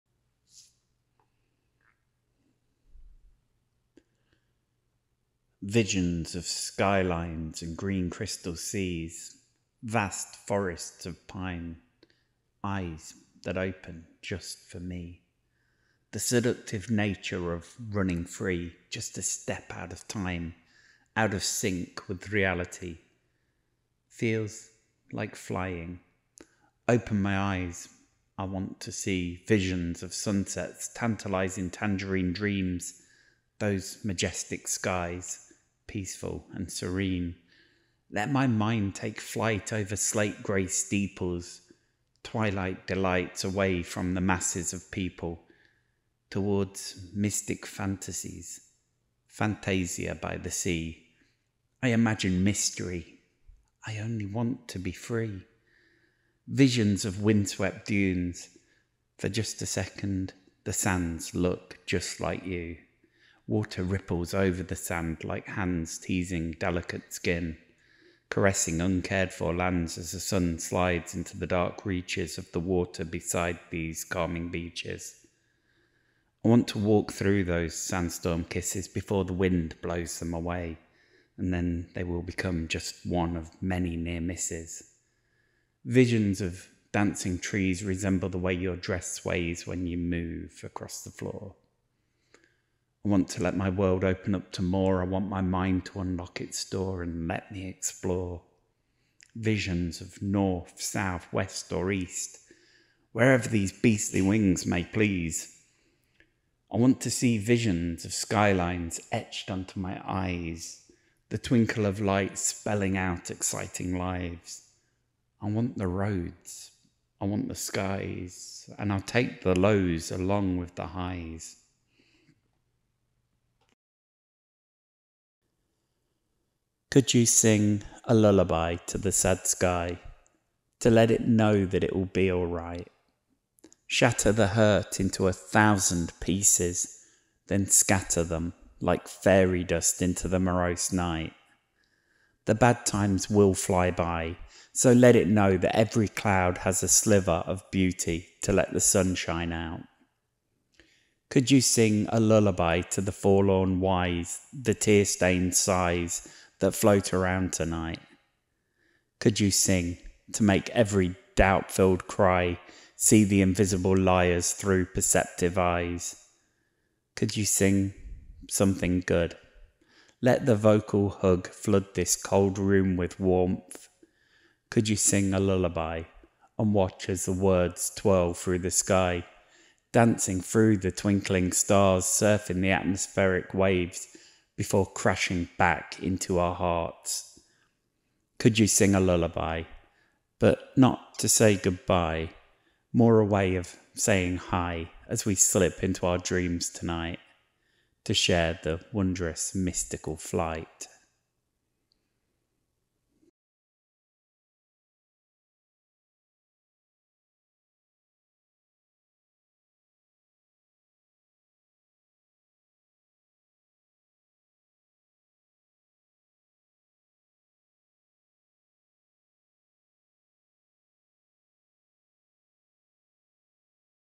Kyle Coare - Visions of Phantasia & Could you sing a lullaby?
Kyle Coare (A Severe Case of Writer's Blog)
Is a Leicester based poet, writer, video editor and spoken word performer.

Peace, Love and Poetry